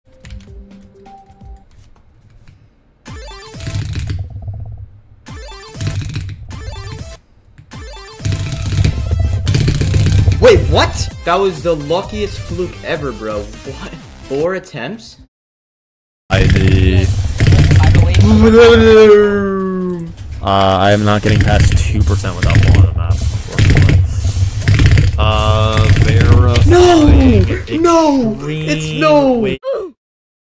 Sayodevice is OP...
I have no idea how I did this. Level is J by Th3Name and the ID is 65309647. This is a top 75 spam challenge by the way.

Gear:
- Keyboard: Ducky One 2 Mini 60% Yellow Switches
- Sayo Device (this is the one I click with usually): Motospeed K2